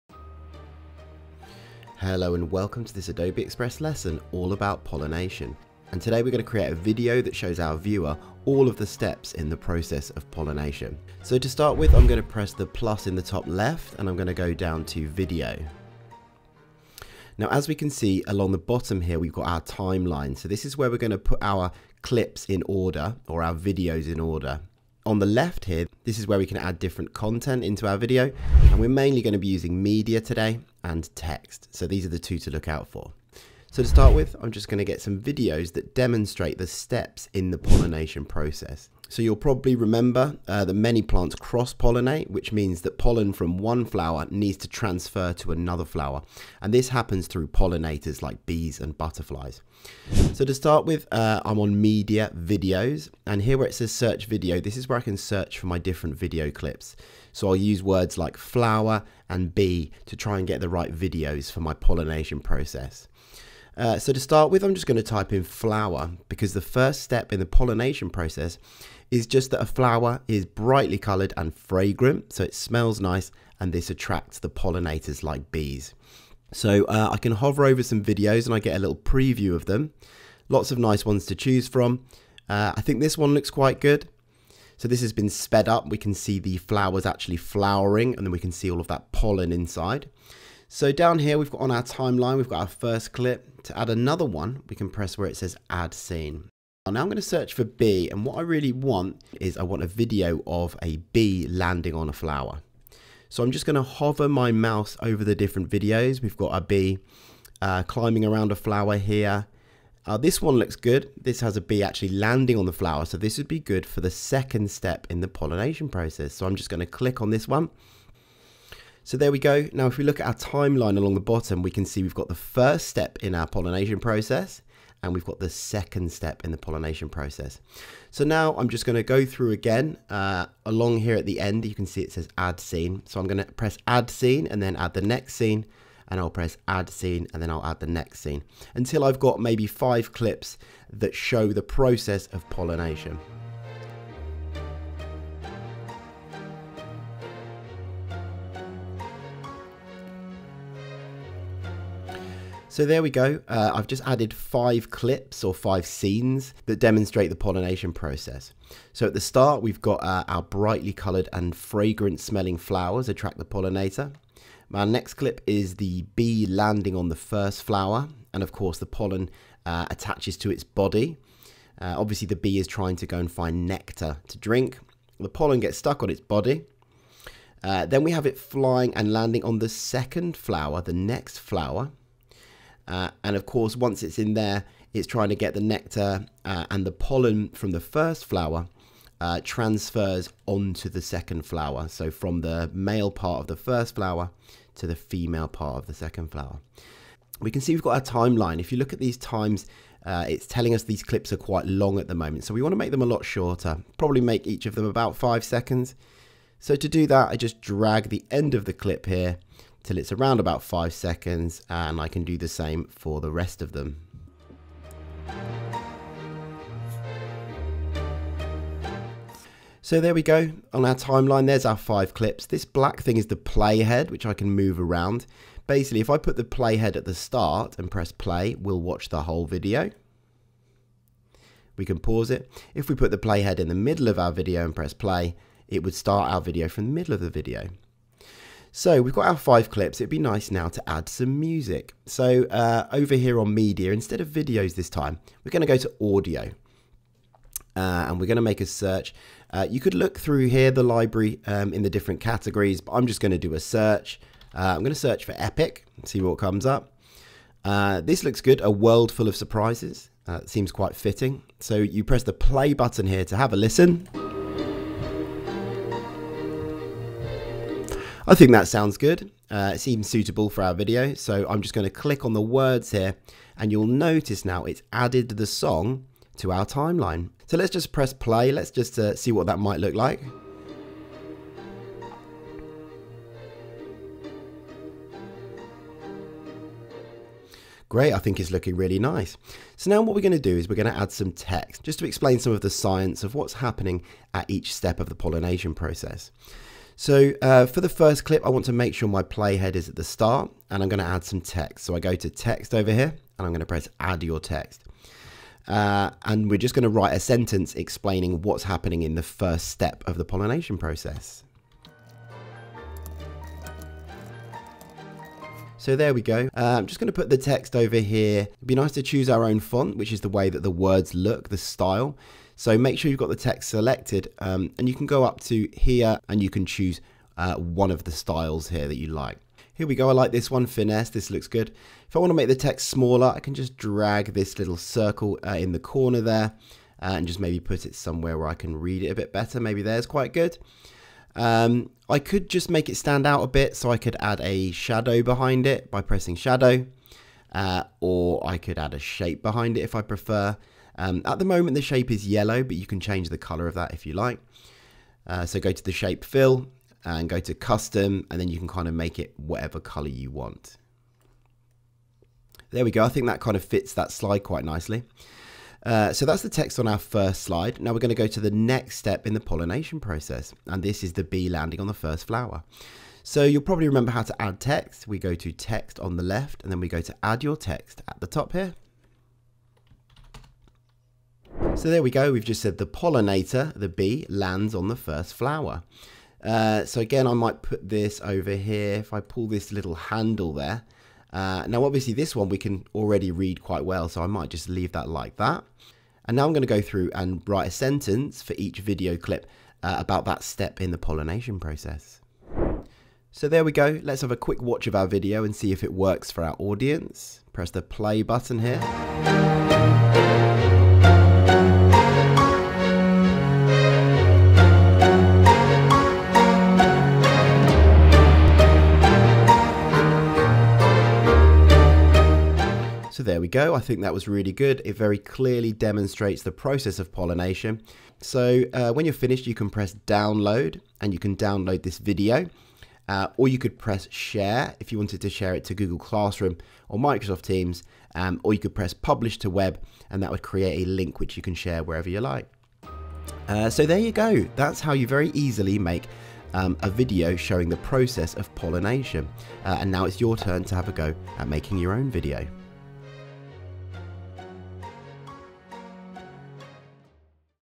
Pollination - Adobe Express Lesson Plan
In this lesson, pupils will create a video about the pollination process.

Learning Objectives

➔ To sequence video clips that demonstrate the steps in the pollination process
➔ To add text that explains each step
➔ To select appropriate music

Cross-curricular Links

Science - Plants / Pollination (KS2)
Computing - Information Technology & Digital Literacy (KS2)

Resources